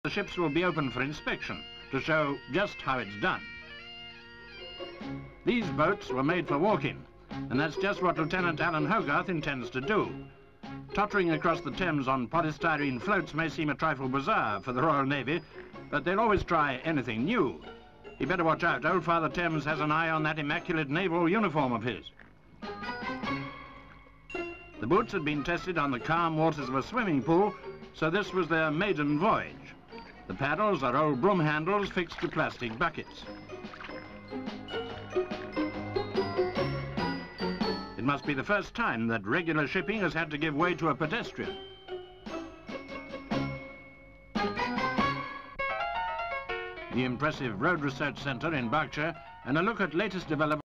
WALKING ON THE THAMES
(24 May 1971) The Navy in a rather bizarre action this time - walking on water. An officer takes a stroll under the Tower Bridge in specially designed 'water boots'.